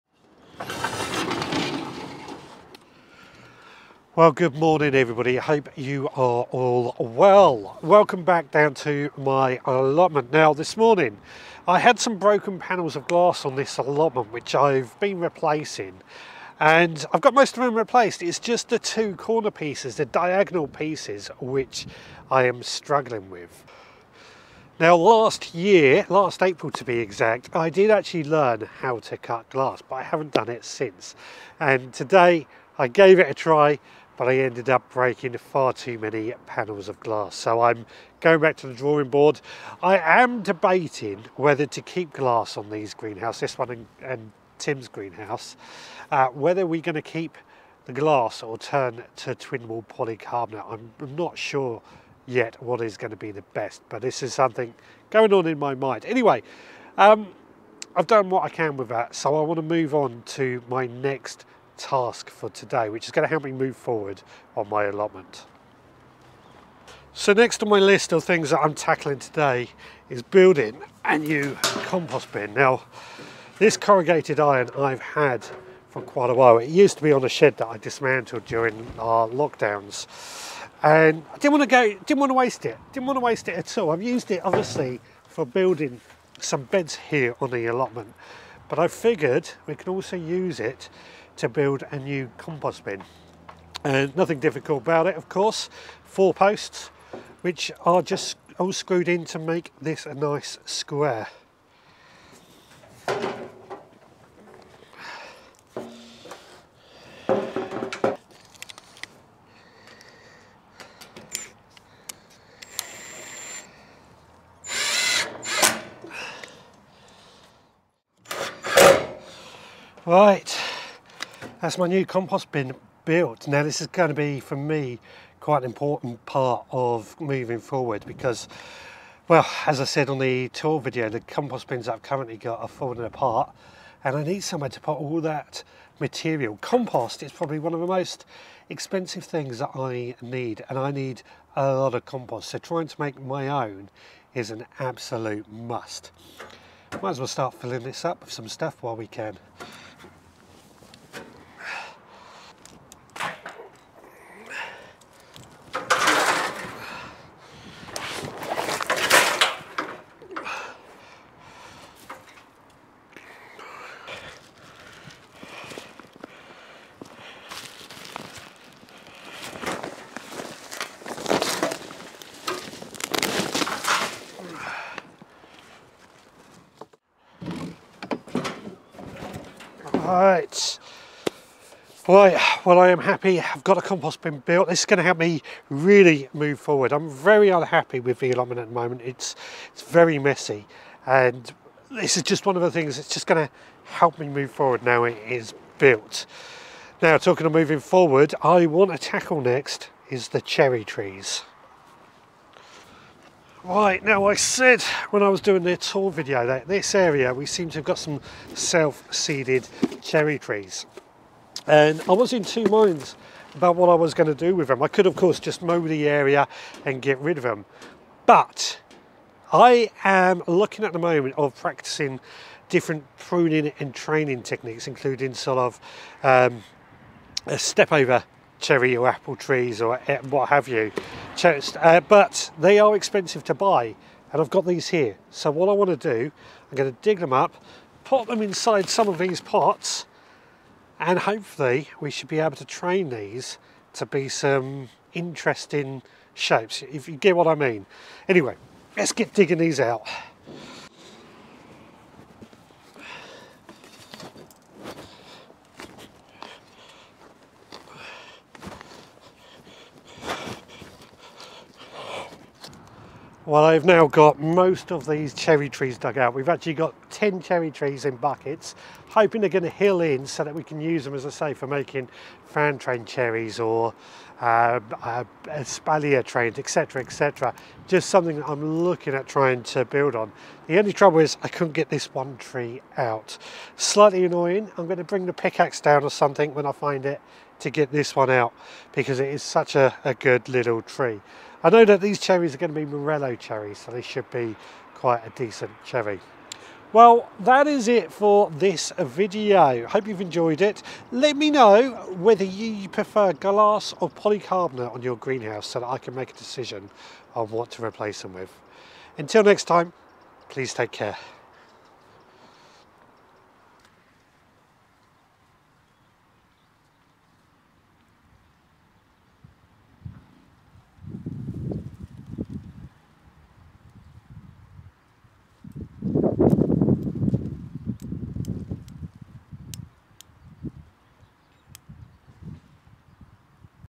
Allotment Update: Replacing Glass, Building Compost Heap & Clearing Self-Seeded Cherry Trees
Join me on the allotment today as I tackle three projects. First, I'm starting to replace the glass on my greenhouse that was damaged in a storm. Then, I'm building a new compost heap to improve the soil for next year's crops. Finally, I'm digging out some self-seeded cherry trees that are taking over a corner of my plot. It's hard work, but I love being out here in the fresh air and getting my hands dirty. Come along for the ride and see what progress we can make!

to find out more about what I do including our weekly podcast, blog and recipies